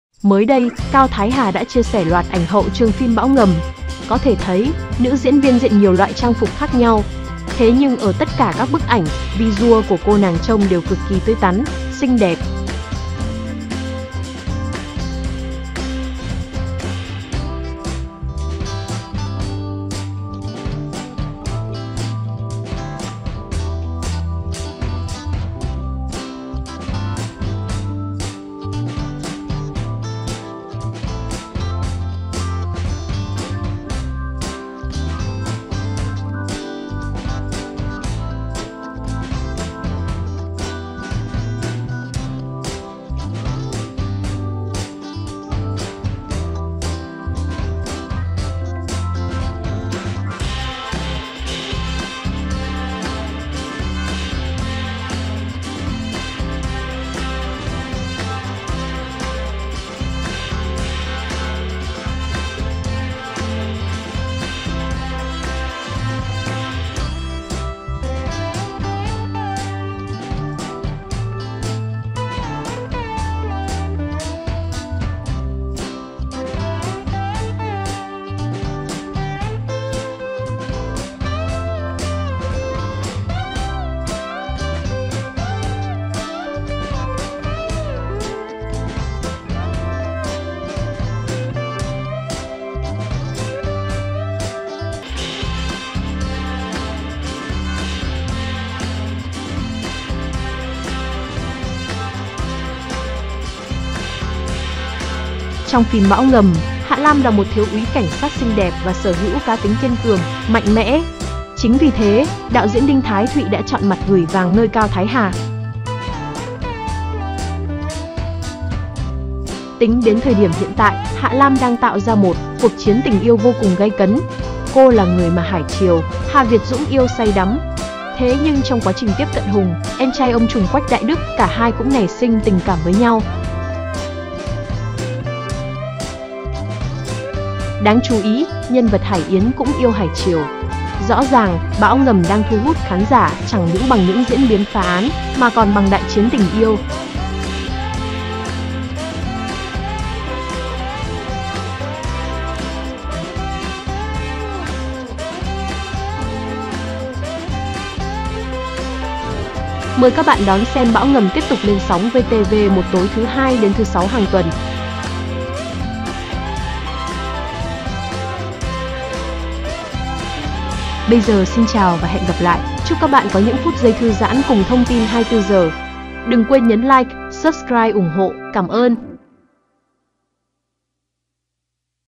Bão ngầm: Hạ Lam - Cao Thái Hà "xinh xỉu" ở hậu trường, bảo sao chả có "đại chiến tình yêu" cực căng
Những hình ảnh cực xinh đẹp của nữ diễn viên Cao Thái Hà ở hậu trường phim Bão ngầm.